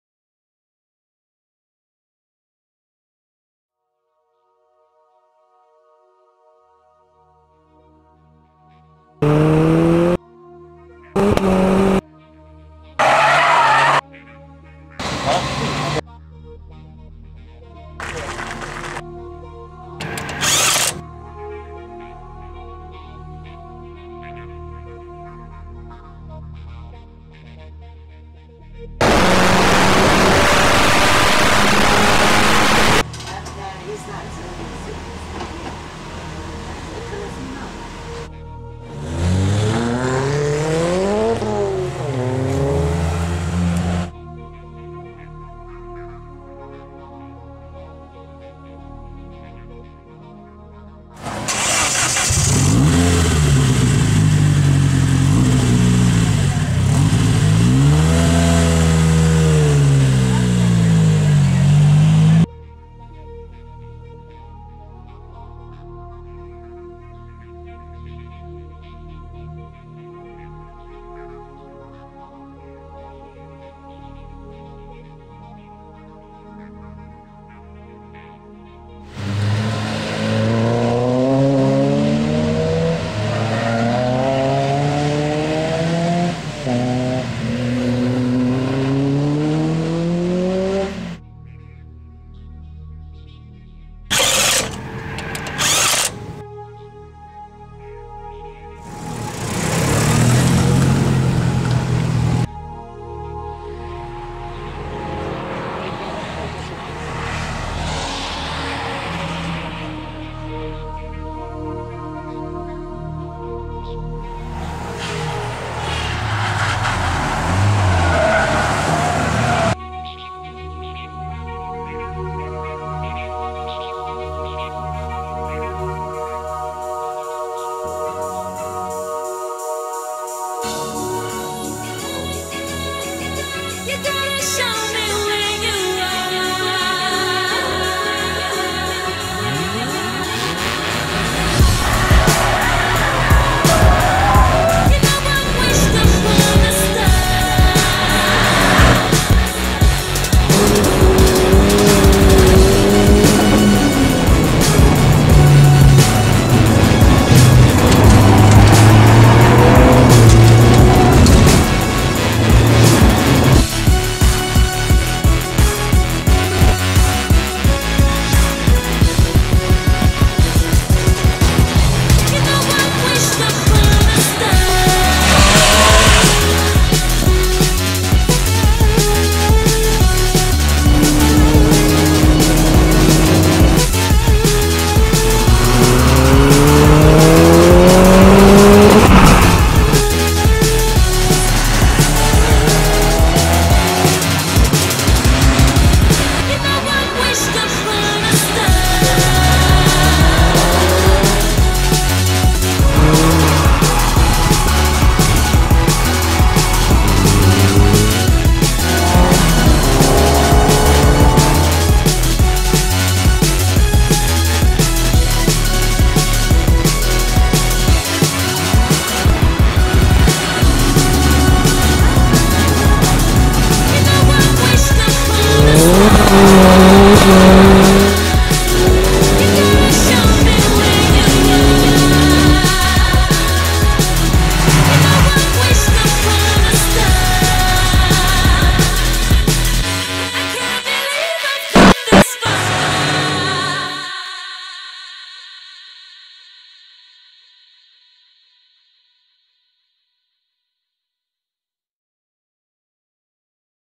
Neo Brothers (unofficial) Trackday film 2011, Cadwell Park.
My edit of the epic day at Cadwell Park Circuit with Neo brothers as I saw and experienced it.